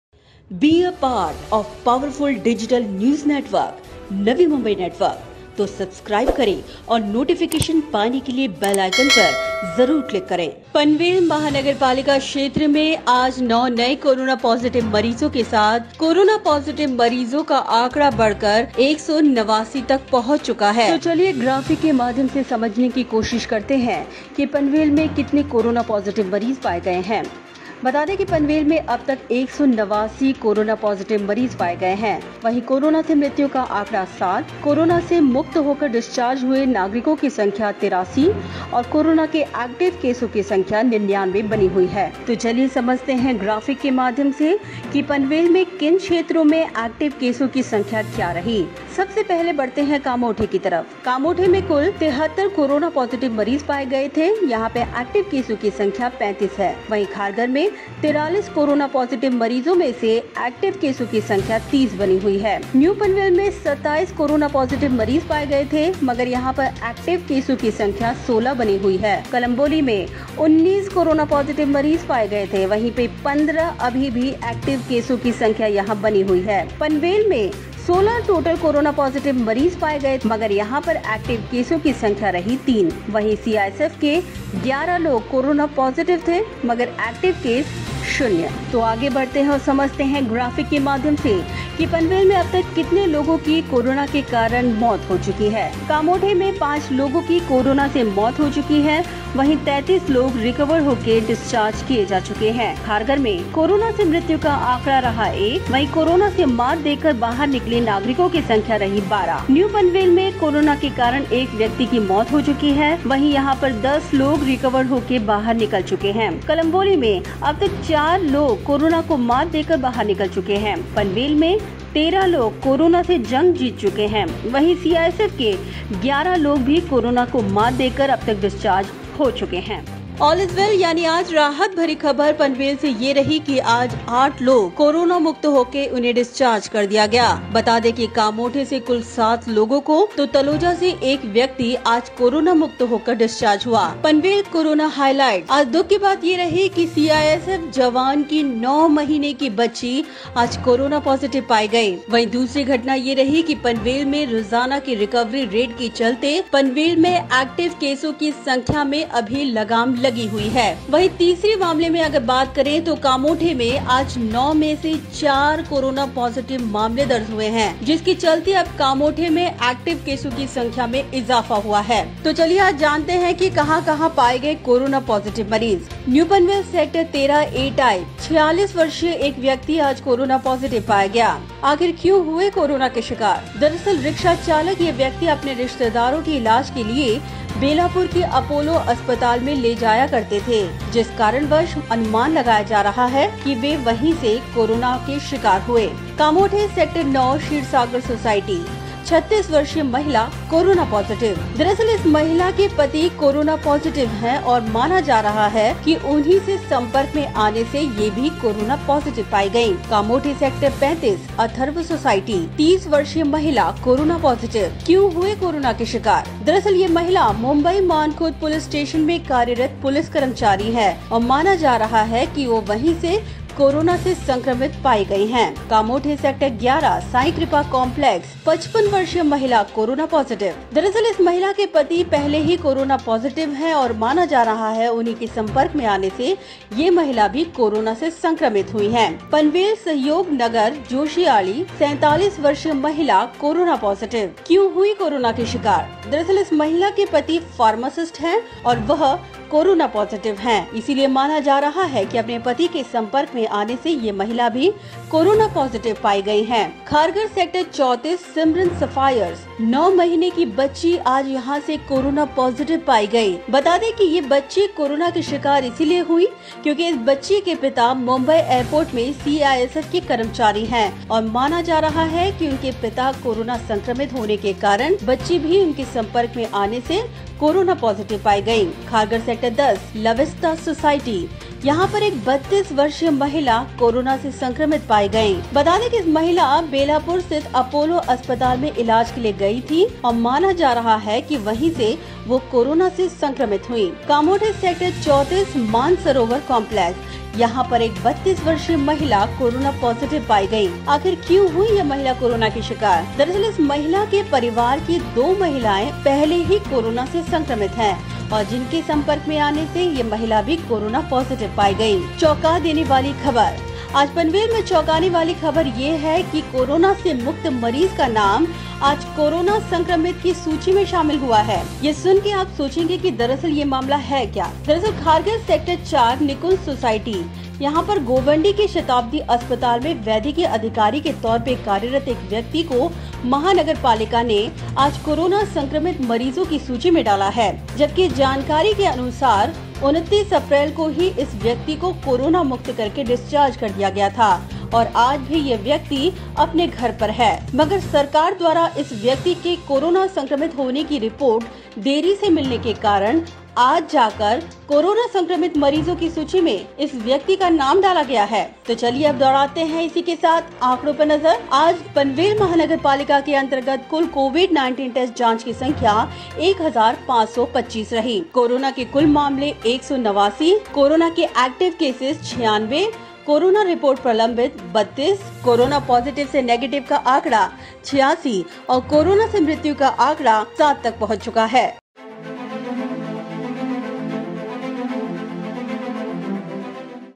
12 May Update:CISF personnel 9 month old baby test positive for Covid-19 in Panvel | Total Cases 189
12 May Update:CISF personnel 9 month old baby test positive for Covid-19 in Panvel | Total Cases 189